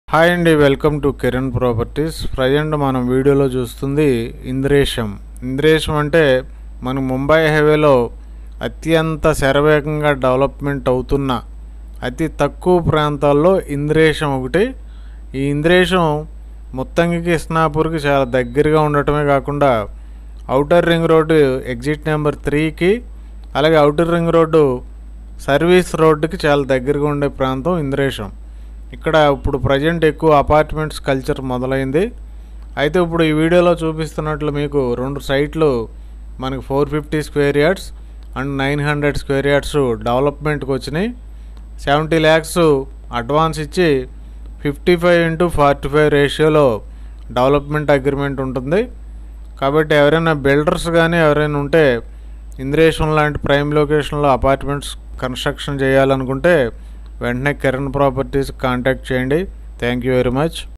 Development Lands for Apartments in Indresham 9063831413 Mumbai Highway Apartments Development Lands
Welcome to Kiran properties. Development Lands in Indresham for Apartments construction near ORR in Mumbai Highway Hyderabad

Land sizes - 450 & 900 Sq Yards

Advance - 70 Lakhs

Ratio - 55/45